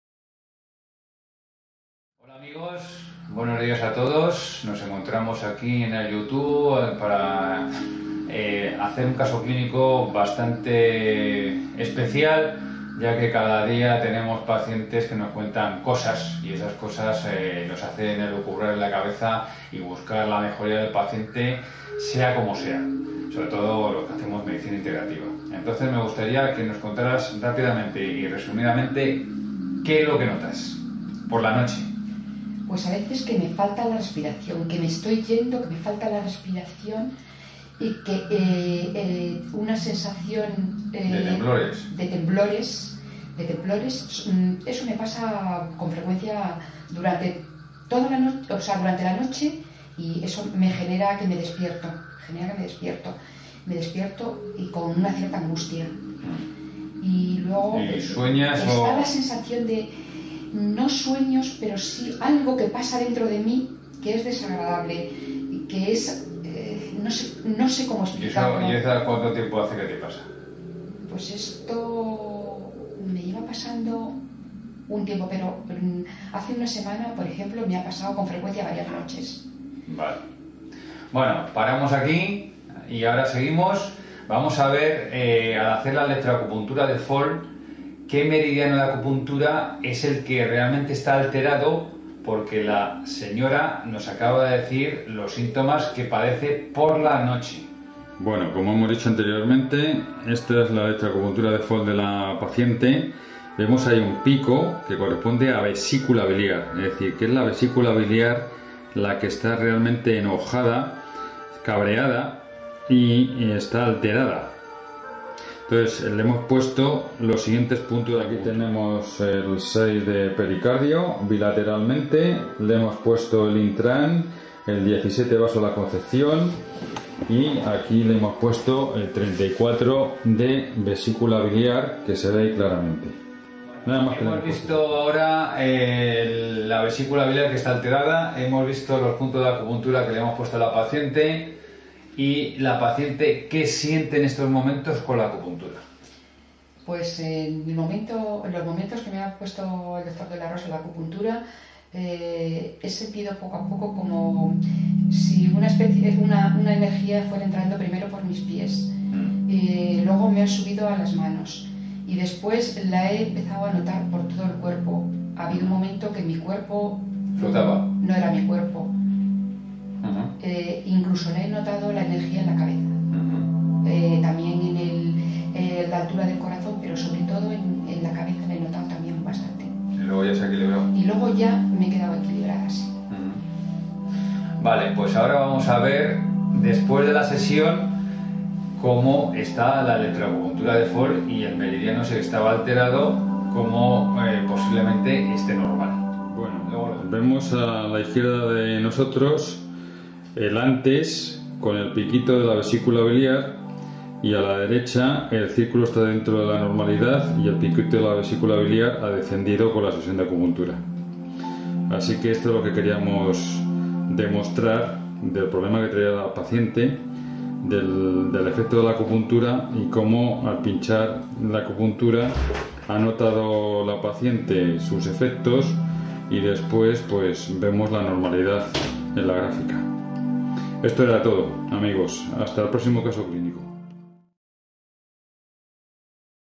ACUPUNTURA: Emociones se dejan medir...
Paciente que acude a nuestra consulta y nos relata sus problemas, medimos, tratamos con acupuntura y volvemos a medir para ver la diferencia.